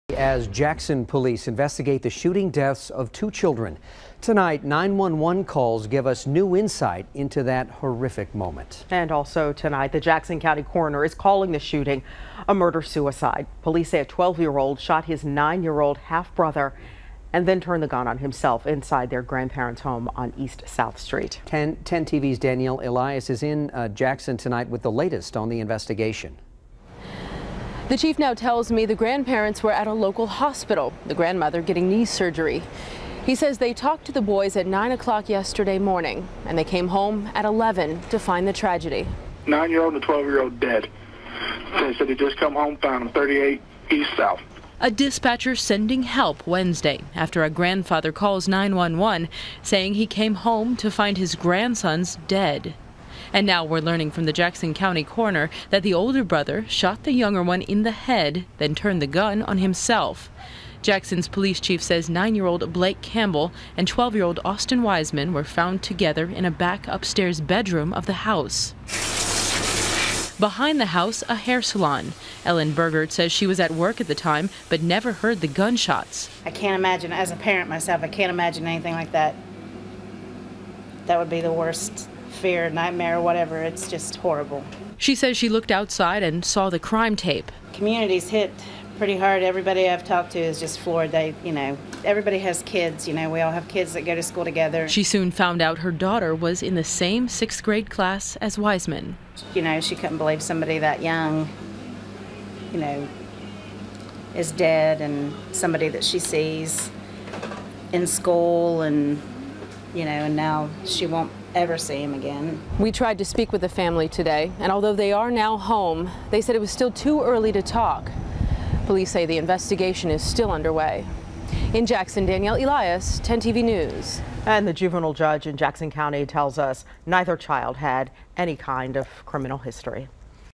Boys' Murder-Suicide
A 12-year-old boy killed his 9-year-old brother, then killed himself.